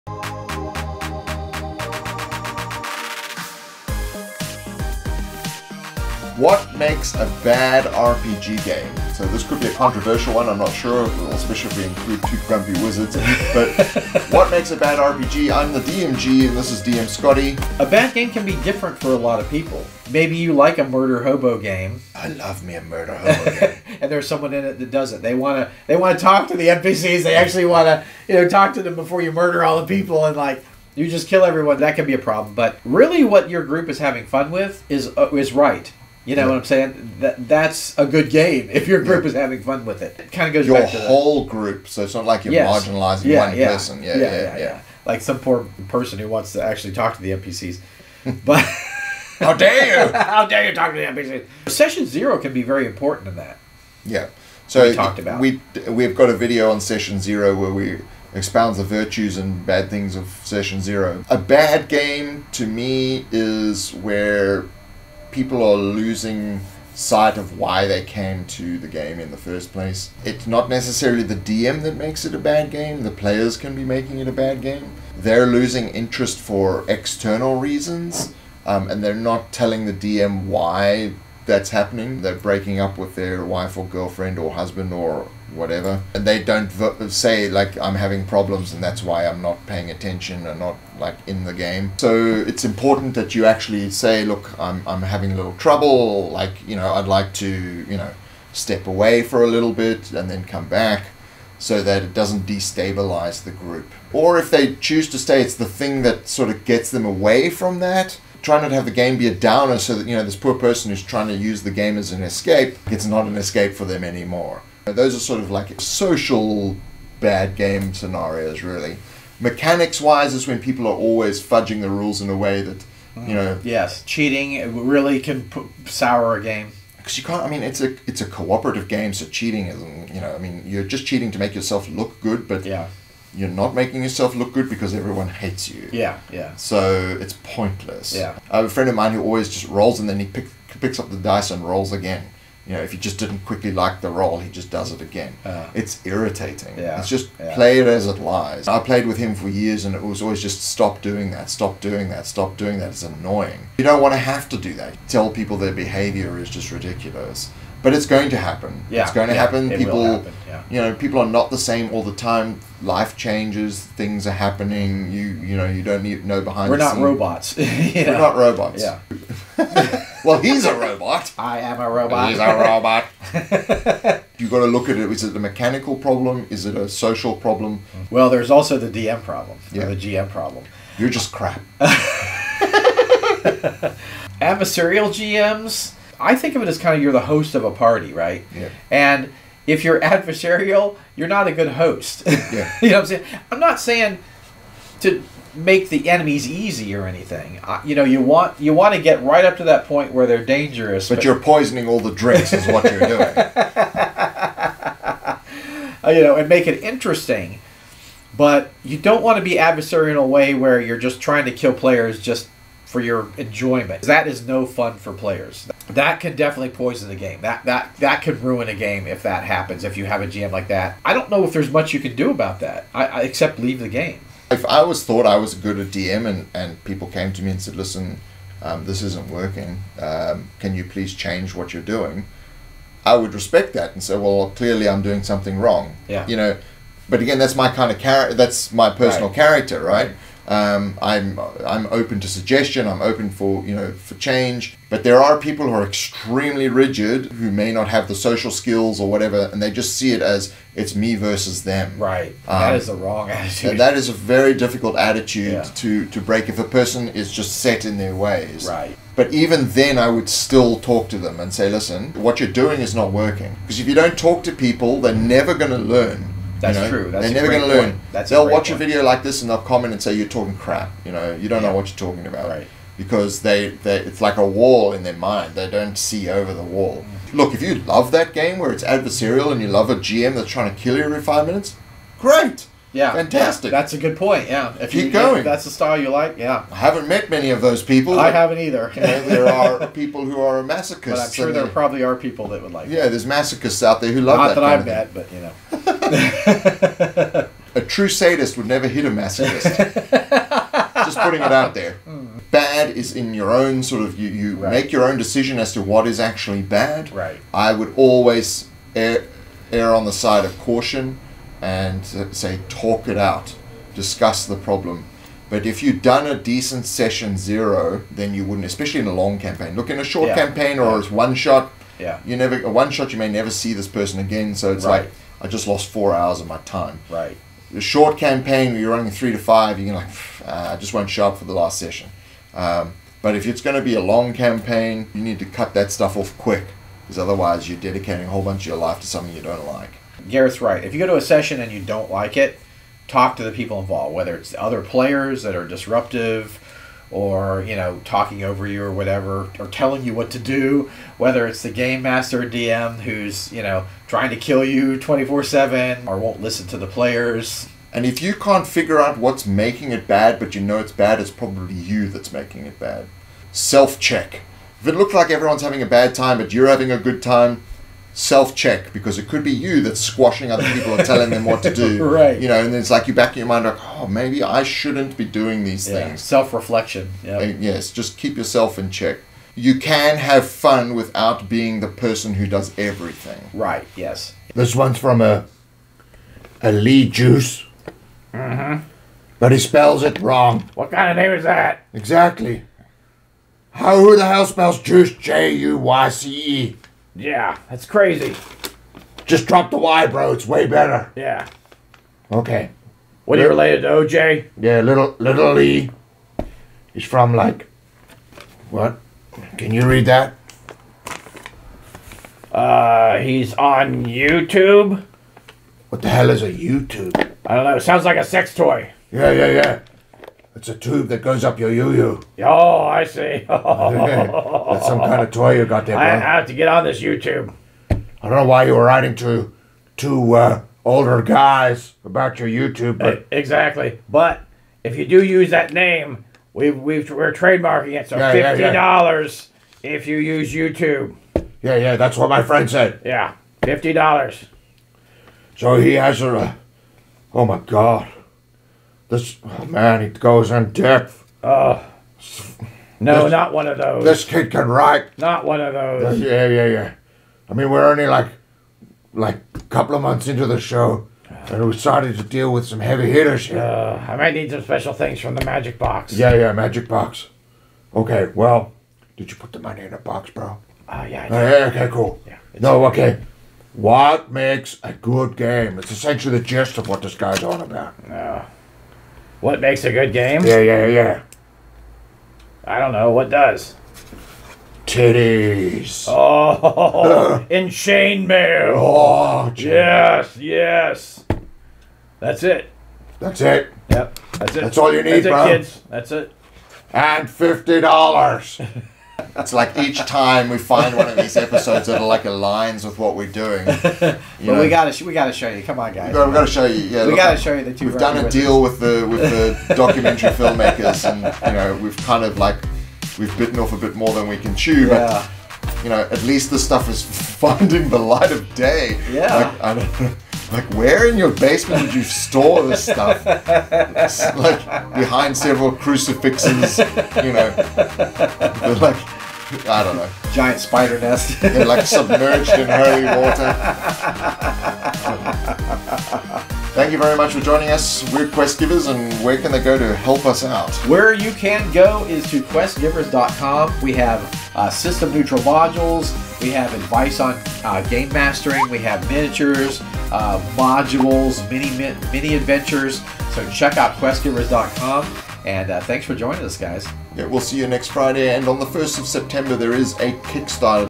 BAD RPGs - What makes a session bad?
How do you know if your RPG session is bad? Playing D&D should be a fun sociable experience - are you having fun?